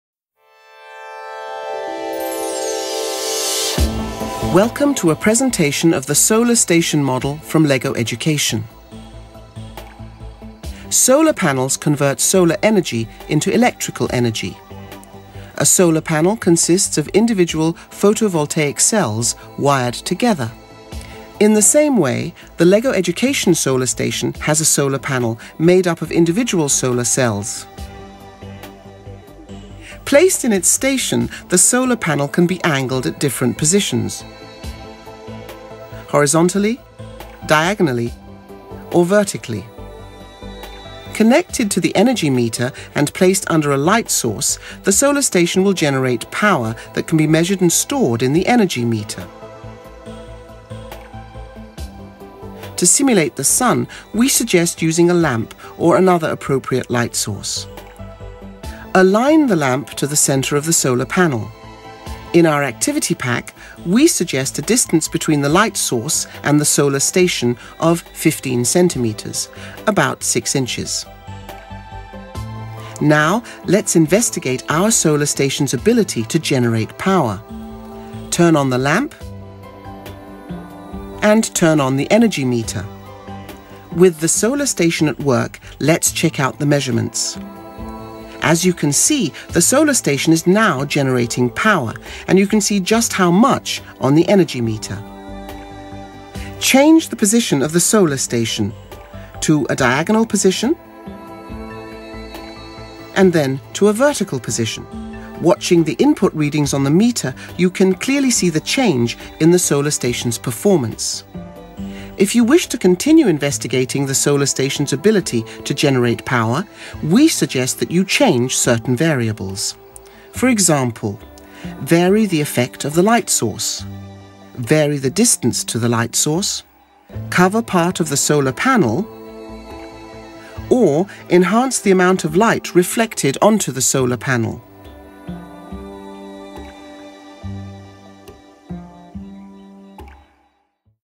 LEGO 9688 Renewable Energy Add-on Set - M&M Solar Station
Intro video for the LEGO Renewable Energy Add-on Set (9688).

Machines & Mechanisms - Solar Station